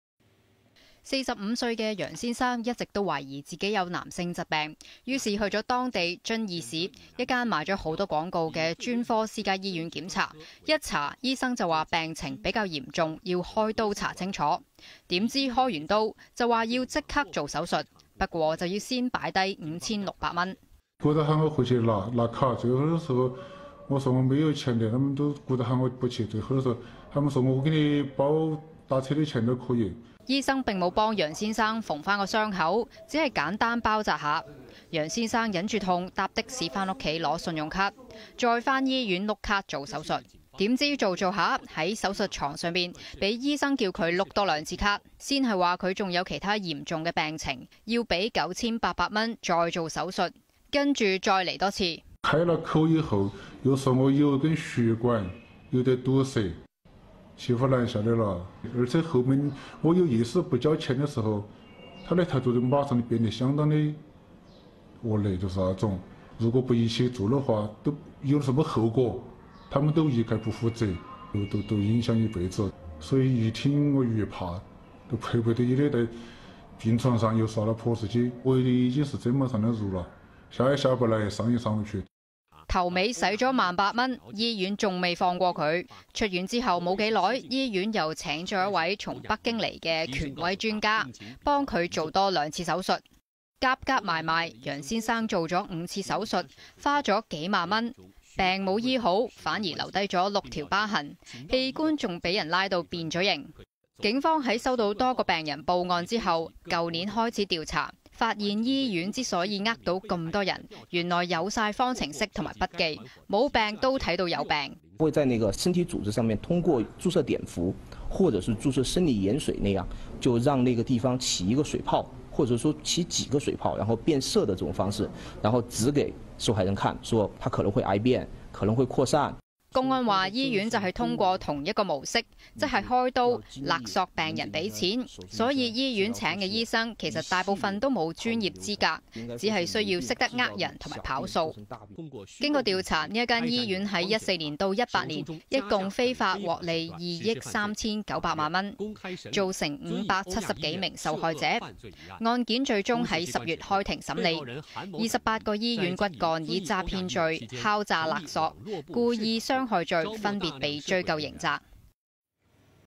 男科手術中途醫生叫加價；病人未縫傷口 飛的趕回家拿錢 - 20191122 - - 有線中國組 - 有線新聞 CABLE News
45歲的楊先生一直懷疑自己有男性疾病，於是去到當地的專科私家醫院檢查。一查之下，醫生說病情比較嚴重，要開刀查清楚，豈料開完刀就說要立即做手術，但要先交5600元。

「就讓我回去拿銀行卡，我說我沒有錢，他們就讓我回去拿錢補齊，他們說給你報銷打車的錢都可以。」醫院並未替楊先生縫合傷口，只是簡單包紮，就讓楊先生忍痛搭的士回家拿信用卡，再回到醫院刷卡做手術。

豈料手術中途，醫生又再叫他刷卡，一開始是說他還有其他嚴重病情，要付9800元再做手術，之後說他還有一根血管堵塞，要再付錢做手術。

「我有意識不交錢的時候，他的態度就馬上變得相當惡劣。他們說，如果不一起做完這些手術的話，有甚麼後果，他們都一概不負責，就影響一輩子。所以越聽我越怕，就迫不得已在病床上又刷卡。我已經是砧板上的肉了，下也下不來，上也上不去。」

這次手術他合共花了18000元，但醫院都還沒放過他。出院後不久，醫院又請了一位來自北京的「權威專家」，幫他做了兩次手術。結果前後做了五次手術，花了幾萬塊，病沒醫好，反而留下六條疤痕，器官還被拉到變了形。

不過，原來受害的不只他一個。警方在收到多個病人報案後，去年開始調查，發現醫院會用各種手段恐嚇病人，沒病說成有病，又在受害人身上注射碘伏，讓他們長水泡，再騙他們有機會癌變擴散，讓他們做手術。

經調查，這間醫院在2014年到2018年，一共非法獲利2.39億元，造成570多名受害者。案件最終在10月開庭審理，28名醫院骨幹以詐騙罪、敲詐勒索罪和故意傷害罪分別被追究刑責。